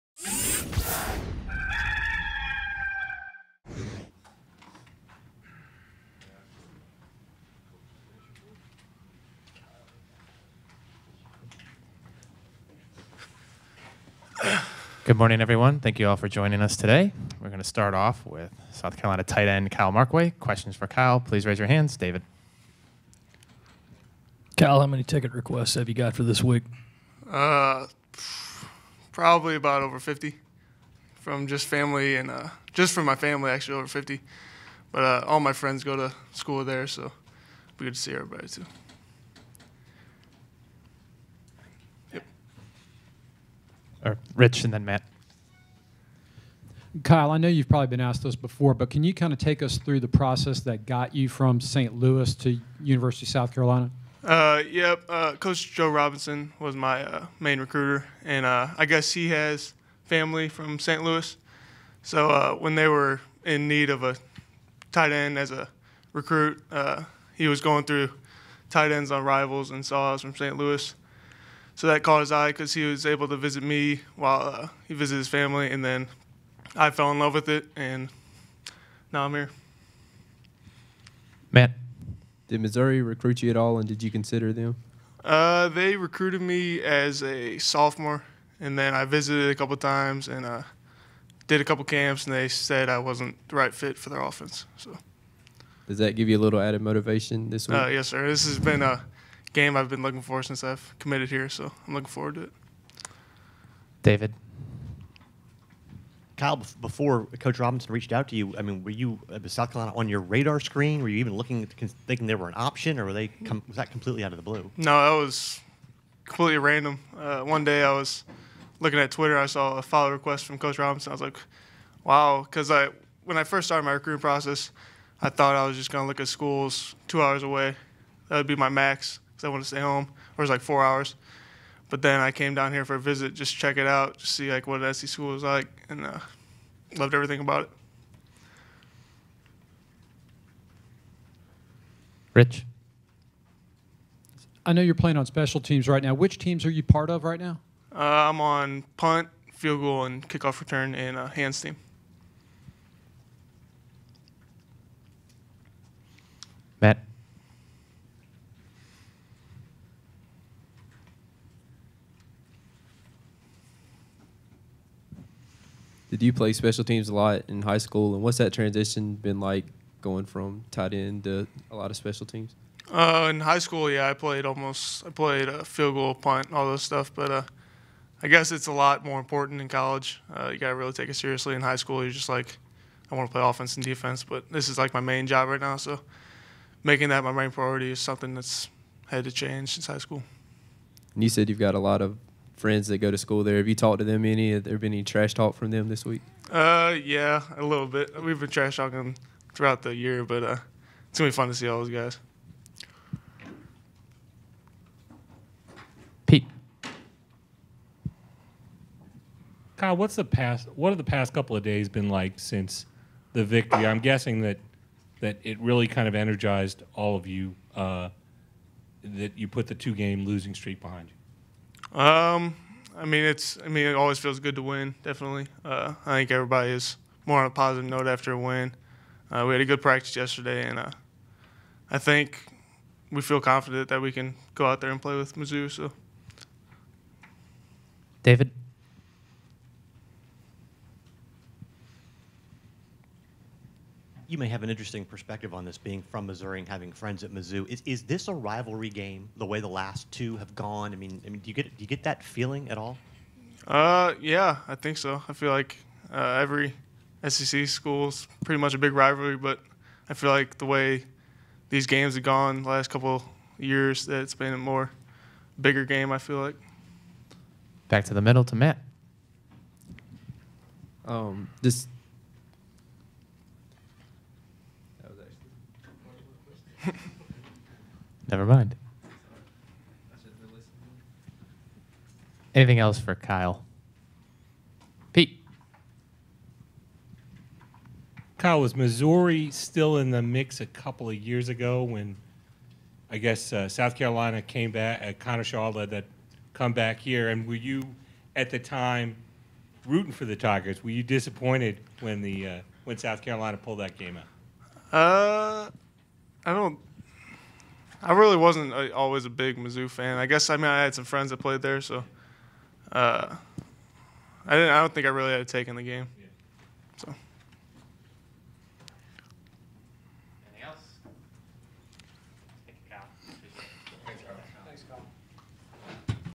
Kyle Markway Press Conference - 9/29/15
Freshman TE Kyle Markway answered questions from the media during the Gamecocks' weekly press conferences on Tuesday, Sept. 29, in the Williams-Brice Stadium media room.